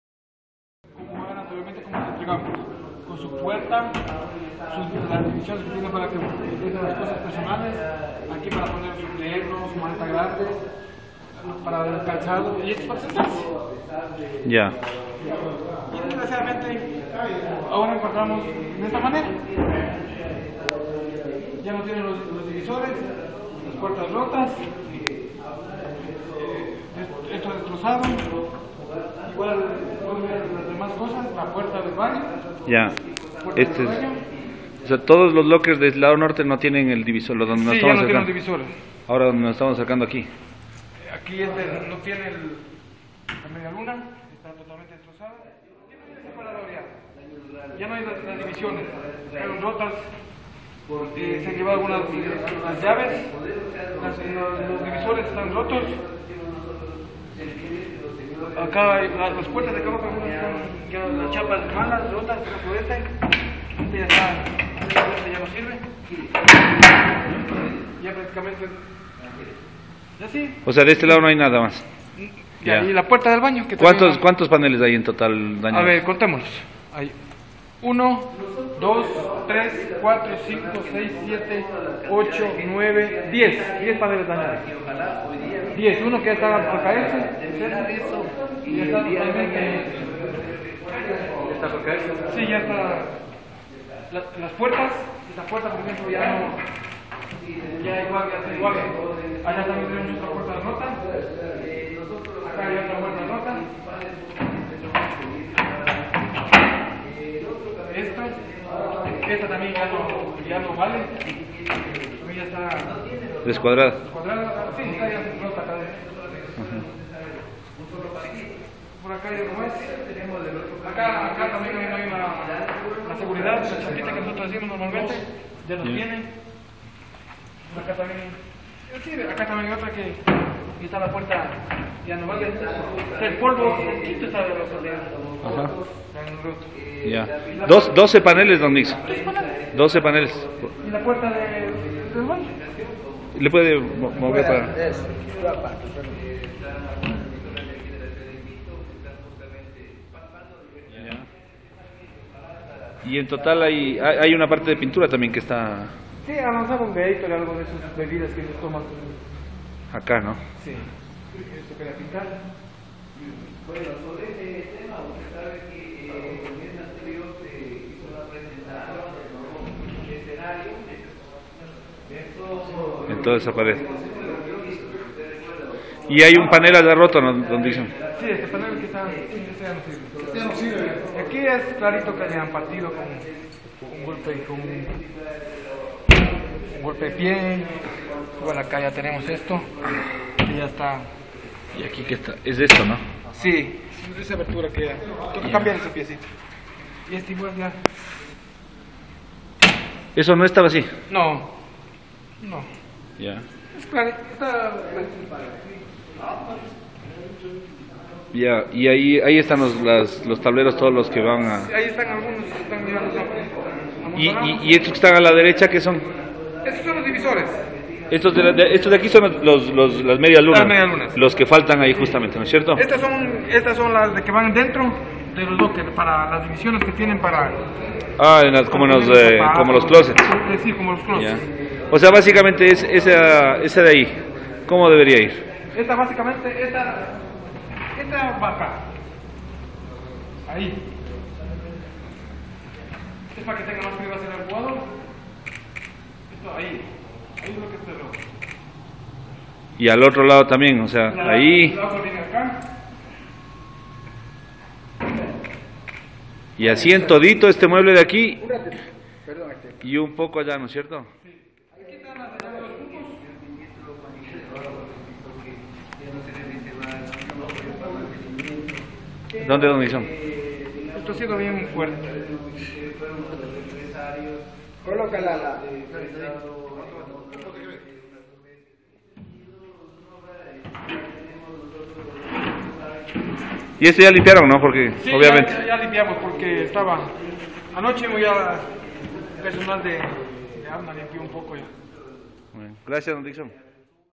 Destrozos camerino de San Lorenzo en el Atahualpa
Tras el empate 1-1 entre Independiente y San Lorenzo, el vestuario del cuadro argentino presentó varios daños en los muebles, puertas y paredes. Les invitamos a certificar los daños en el camerino, gracias a las explicaciones de Sixon Guarnizo, empleado del estadio Atahualpa.